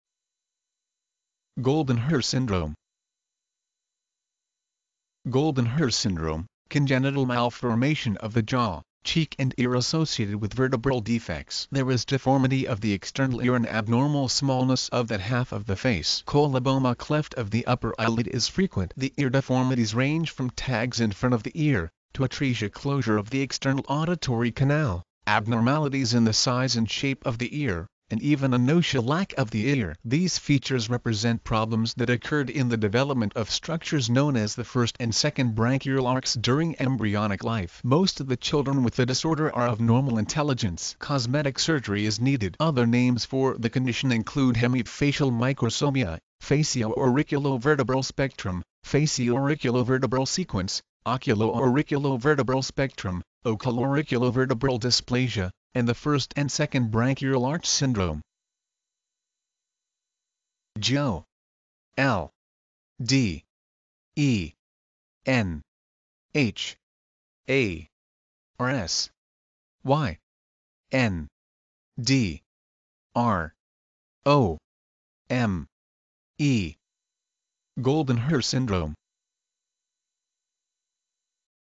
Goldenhar syndrome - Medical Definition and Pronunciation
Title: Goldenhar syndrome

Goldenhar syndrome: Congenital malformation of the jaw, cheek and ear associated with vertebral defects. There is deformity of the external ear and abnormal smallness of that half of the face. Coloboma (cleft) of the upper eyelid is frequent. The ear deformities range from tags in front of the ear, to atresia (closure) of the external auditory canal, abnormalities in the size and shape of the ear, and even anotia (lack of the ear). These features represent problems that occurred in the development of structures known as the first and second branchial archs during embryonic life. Most of the children with the disorder are of normal intelligence. Cosmetic surgery is needed. Other names for the condition include hemifacial microsomia, facio-auriculo-vertebral spectrum, facioauriculovertebral sequence, oculo-auriculo-vertebral spectrum, oculoauriculovertebral dysplasia, and the first and second branchial arch syndrome.